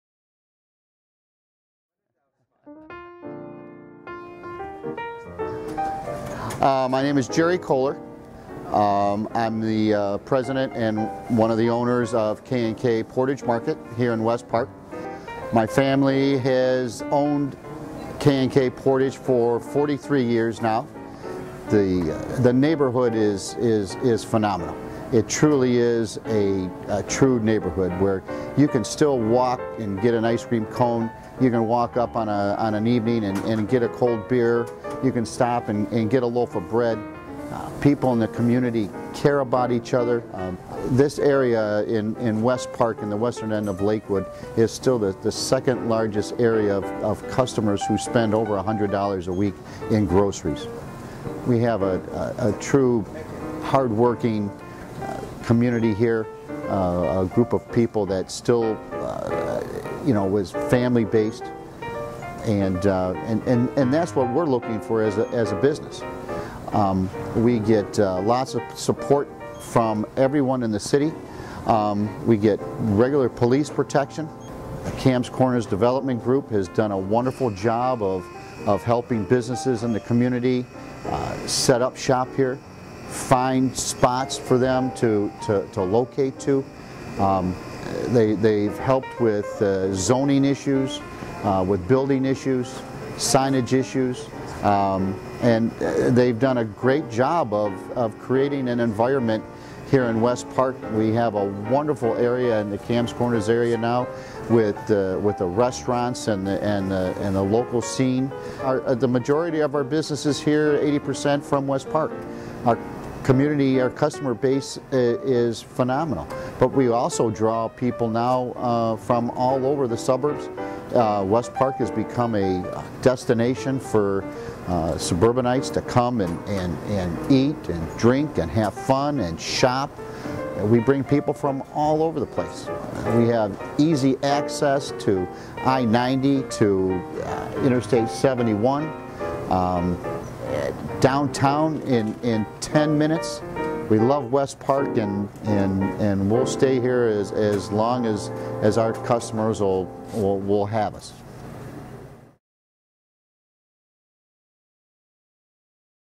K&K Portage Market
Specializing in fresh meats, poultry, deli, seafood, bakery, dairy, imported beer, wine, and freezer meat, K&K Portage Market has been serving Cleveland and the west side communities since 1972.